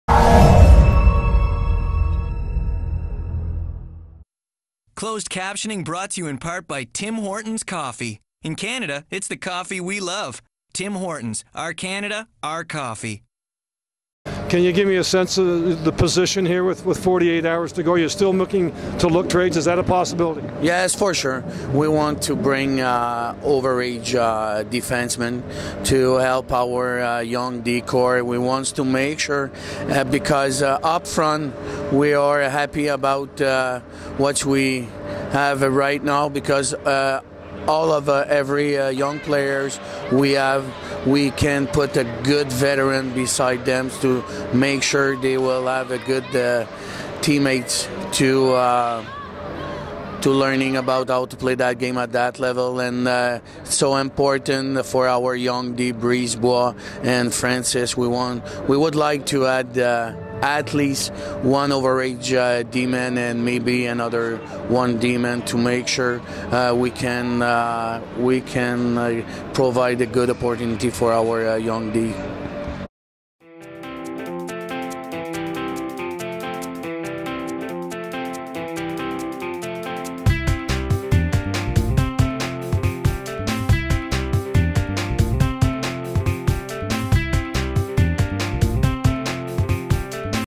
Titan trade deadline
Titan Head Coach Mario Pouliot says his team hopes to acquire a couple of defence men before Tuesday's noon QMJHL trade deadline.   John Moore interviews Pouliot prior to Sundays game in Halifax.